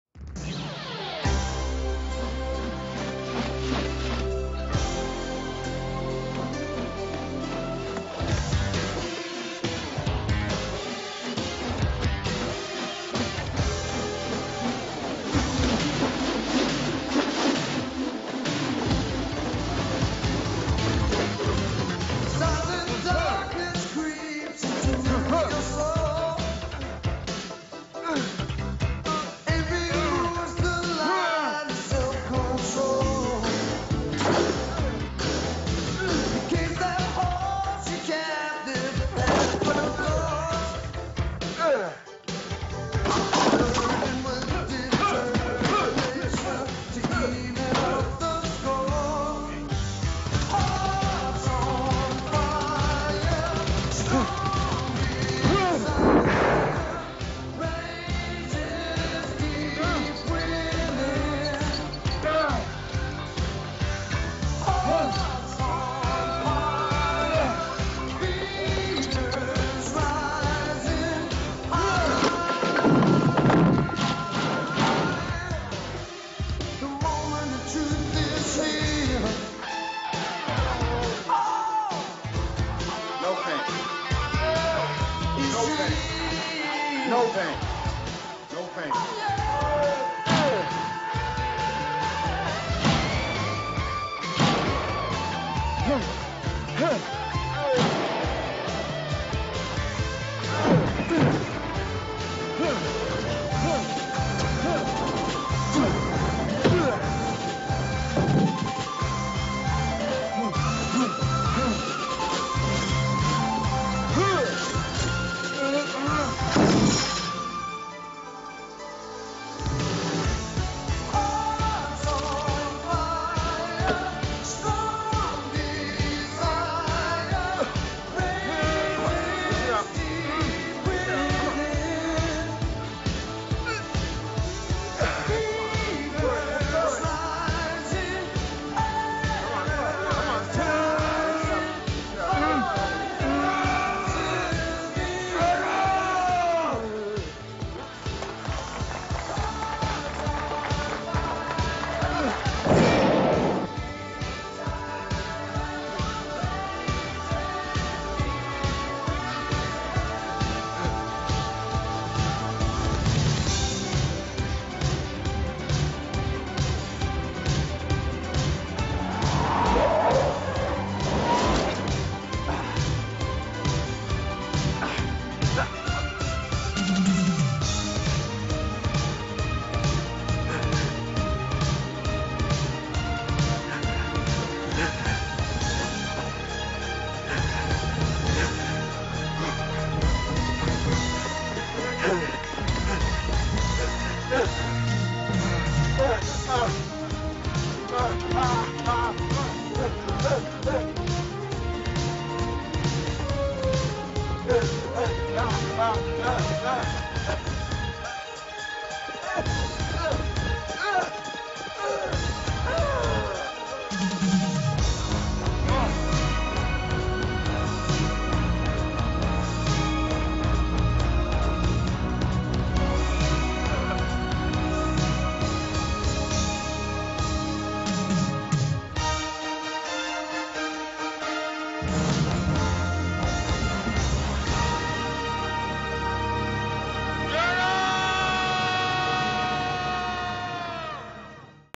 Matt Cross training for his return to Firestorm Pro Wrestling
Matt Cross returns to Firestorm Pro on June 17th for The Rise of The Real Underground 4 at The Livewire in Mentor on the lake, OH.  Belltime 8pm